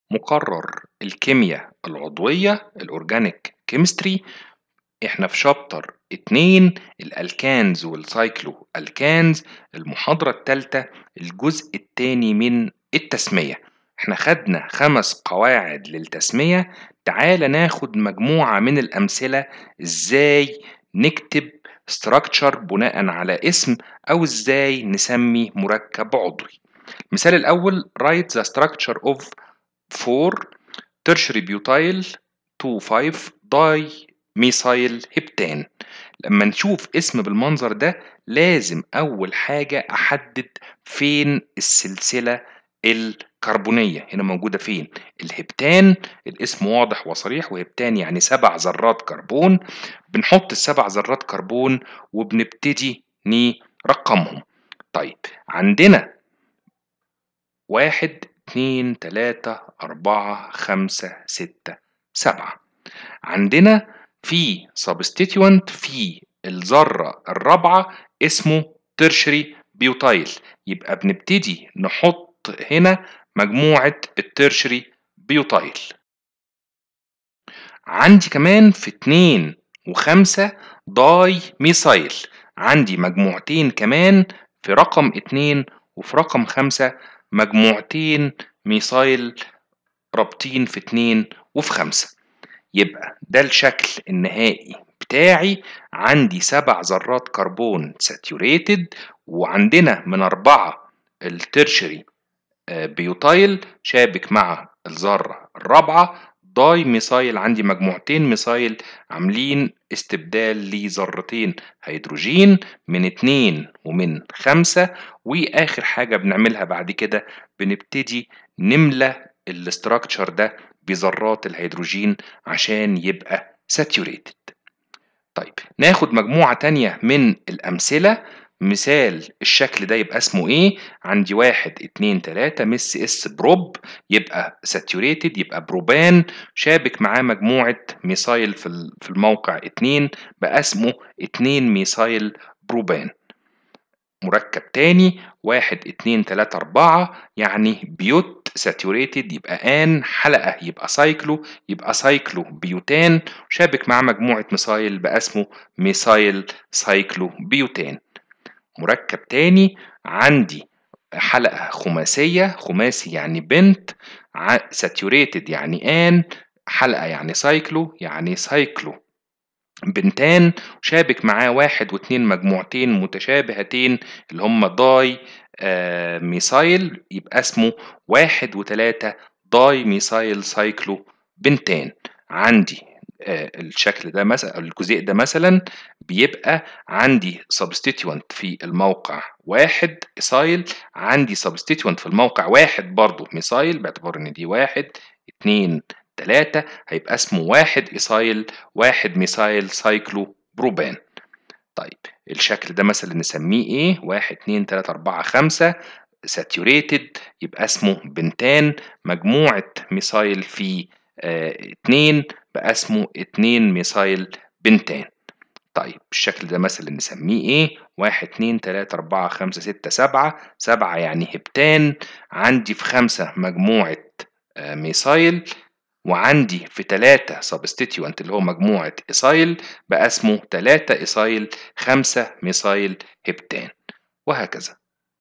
Organic Chemistry - U2-L3 : Nomenclature 2
المقررات المفتوحة
كيمياء العضوية - 111كيم  Organic Chemistry
2nd unit: Alkanes and cycloalkanes
3rd lecture: Nomenclature 2
1- Example (get the structure)
2- Examples (write the name)